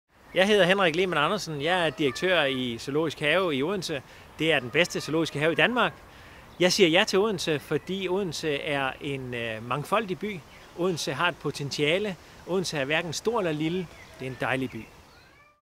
Ja Odense 2
Direktør for Odense Zoo, Henrik Lehmann Andersen, siger Ja til Odense.